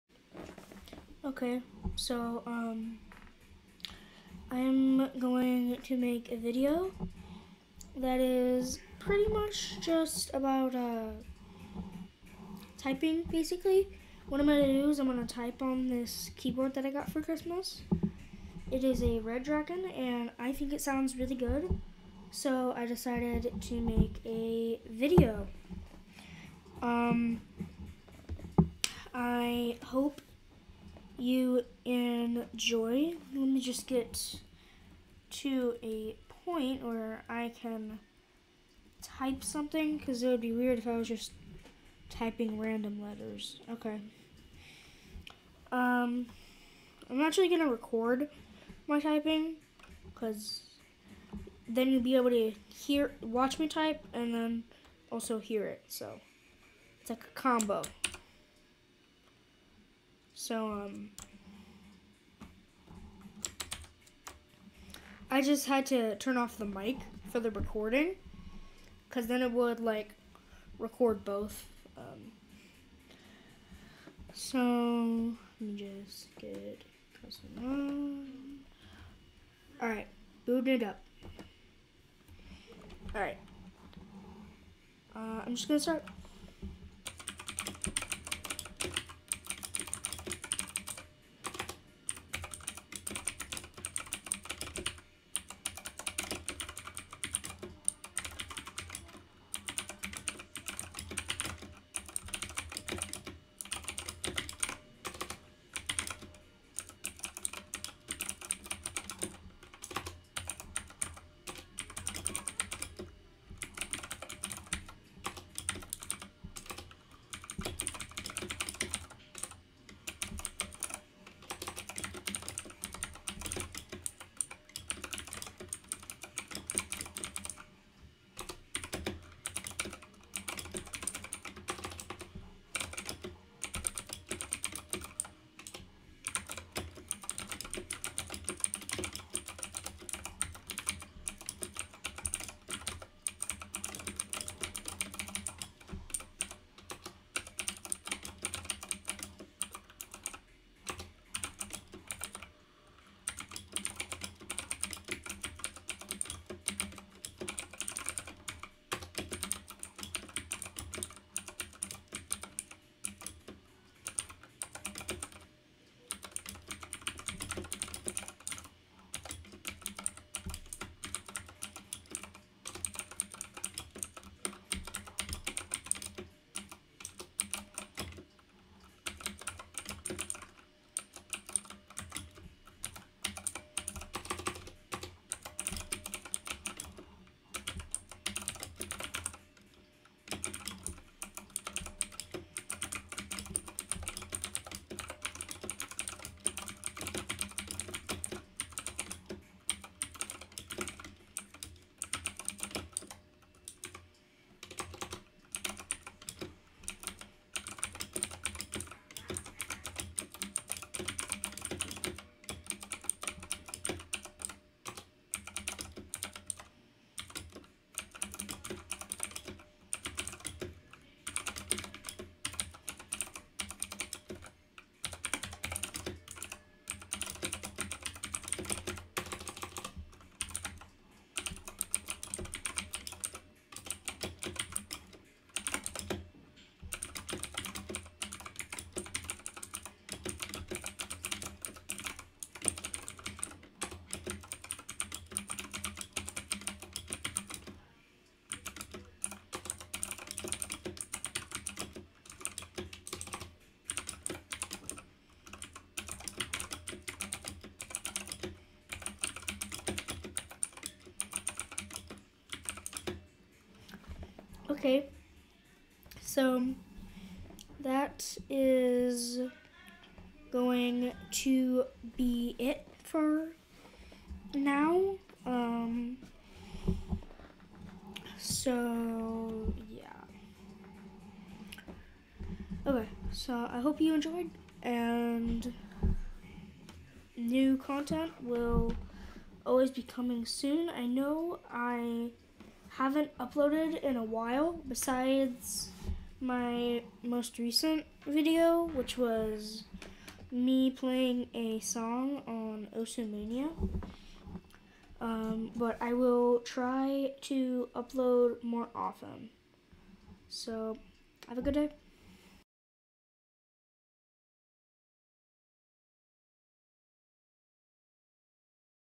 My new Redragon Keyboard!
This is a video where I will be demonstrating some sounds of the new
Redragon Keyboard I got for Christmas.